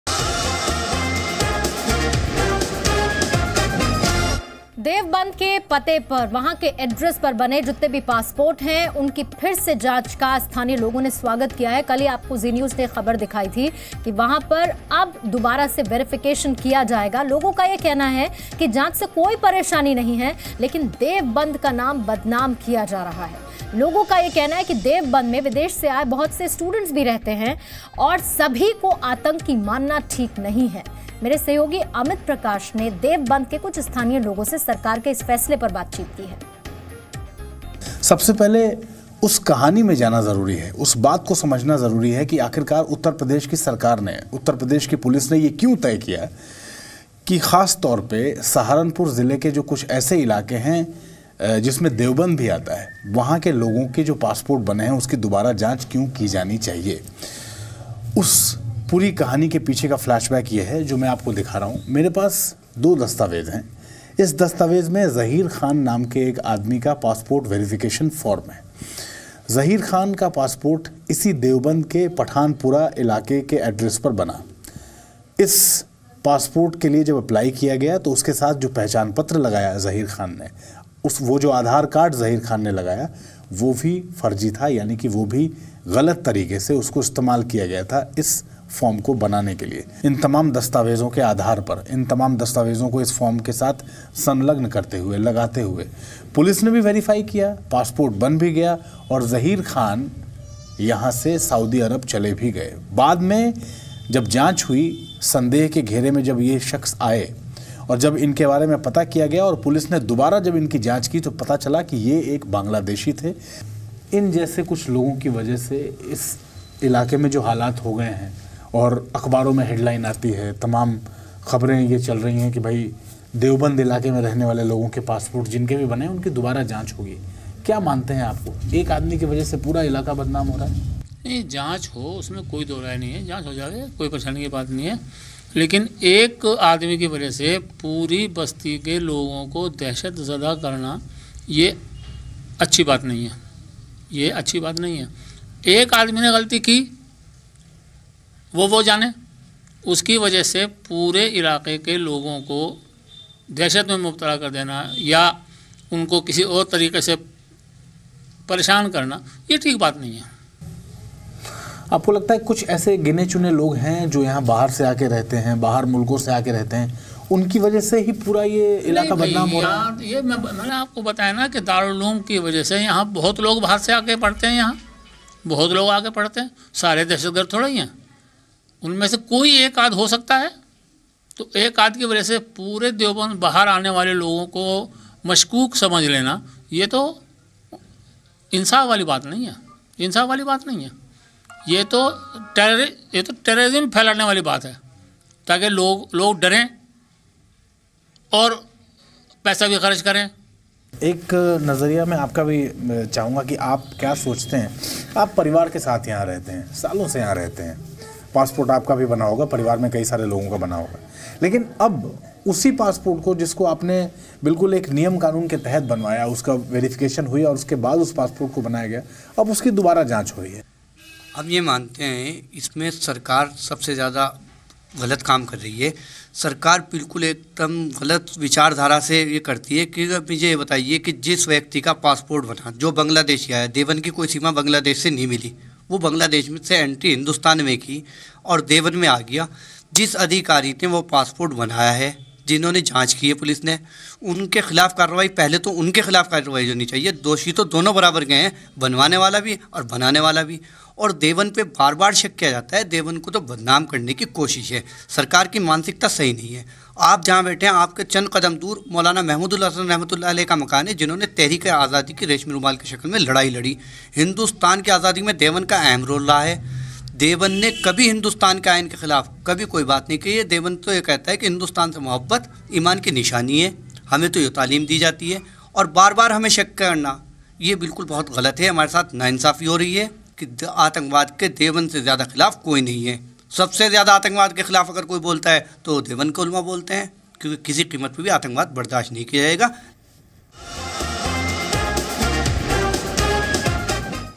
All passports in Deoband to be rechecked after suspected terror links
This segment of Zee News brings to you latest updates on Passport verification in Deoband. Watch this clip to know more.